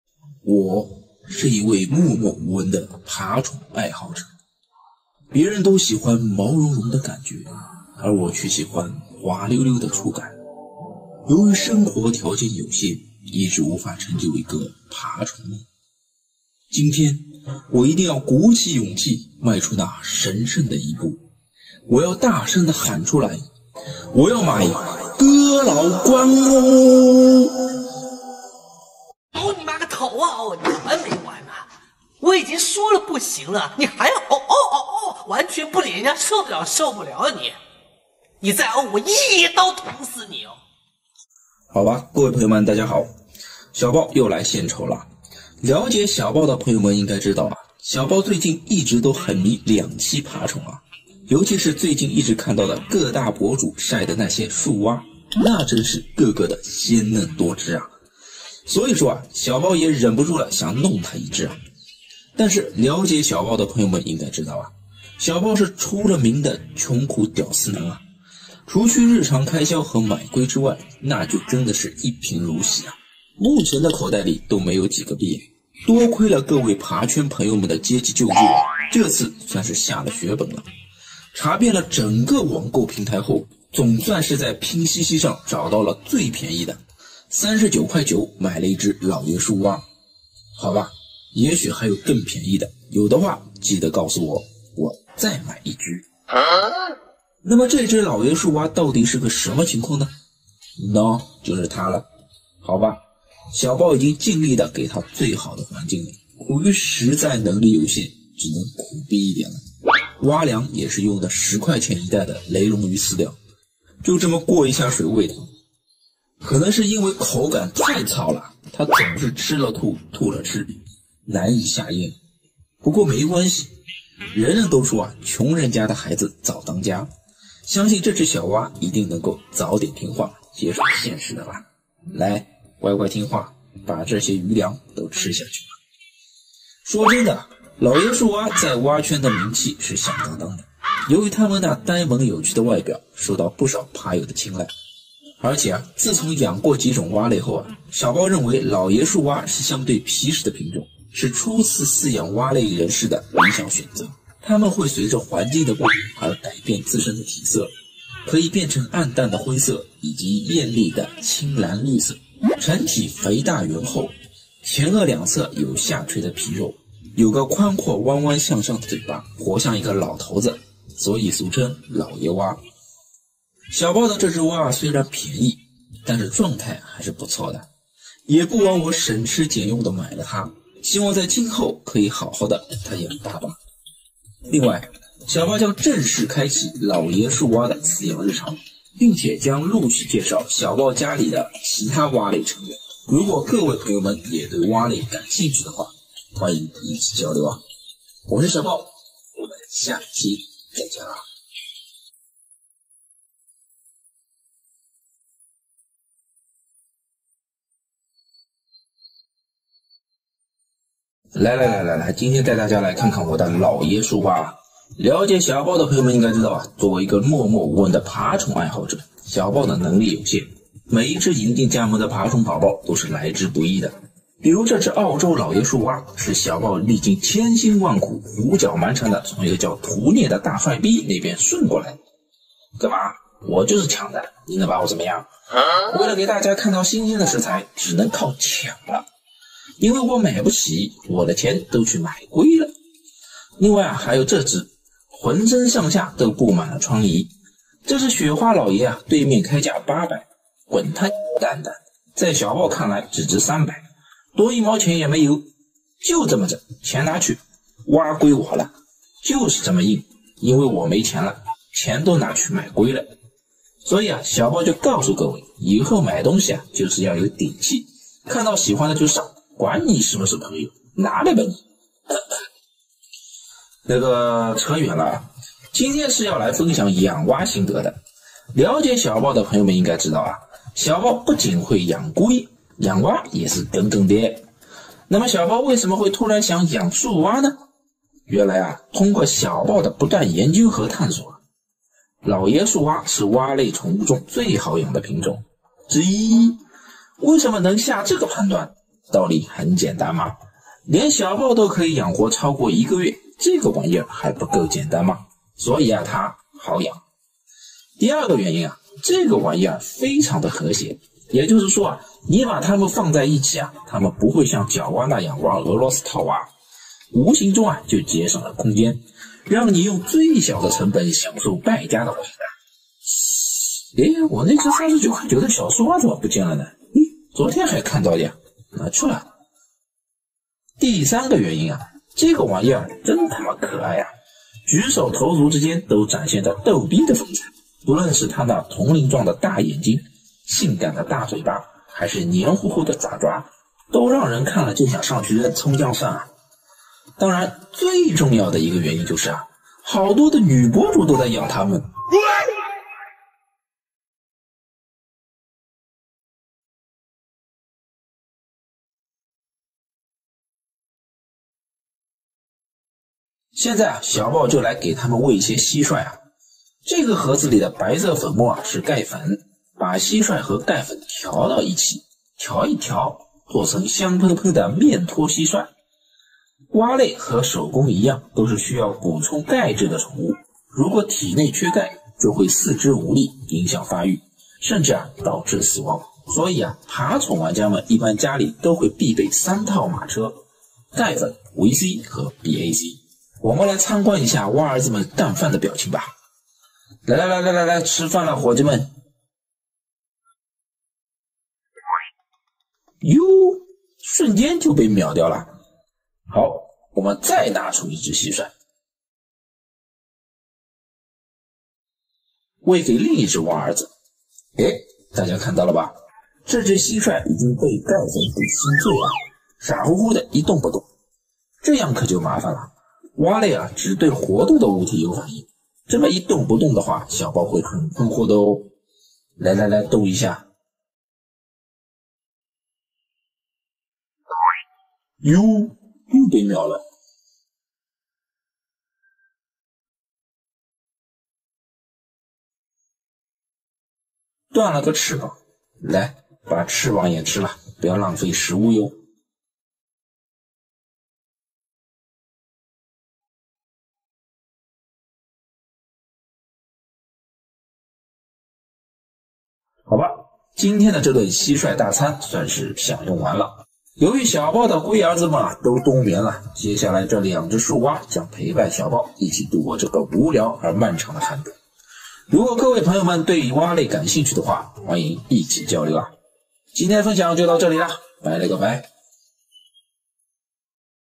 錢都拿去買烏龜了，這只姥爺樹蛙是搶來！給樹蛙寶寶餵蟋蟀
養只龜，慢吞吞，一起過日子
分享養龜的知識以及有趣的故事~😋
跟你分享養龜的每一天！
跟著阿暴，一起養龜~